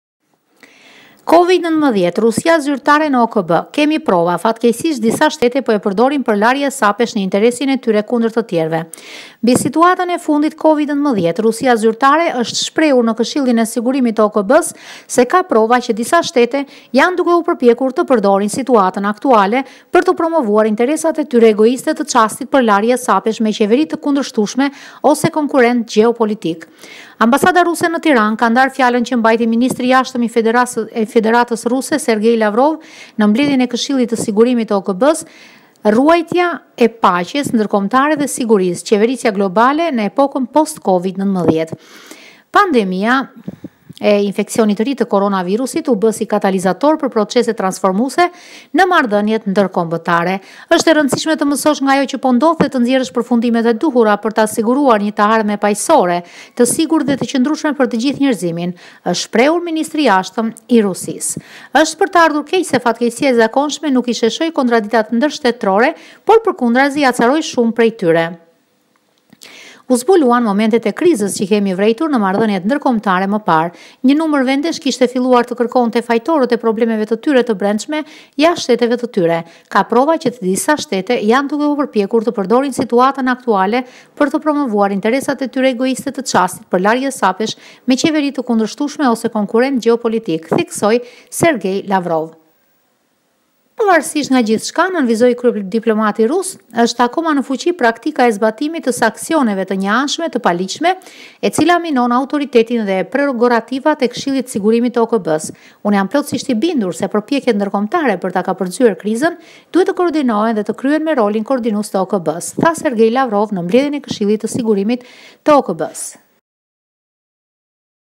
Rusia zyrtare në OKB: Kemi prova, fatkeqësisht disa shtete po e përdorin për larje hesapesh në
Ambasada Ruse në Tiranë ka ndarë fjalën që mbajti Ministri i Jashtëm i Federatës Ruse Sergej Lavrov në mbledhjen e Këshillit të Sigurimit të OKB-së “Ruajtja i paqes ndërkombëtare dhe të sigurisë: Qeverisja globale në epokën post COVID-19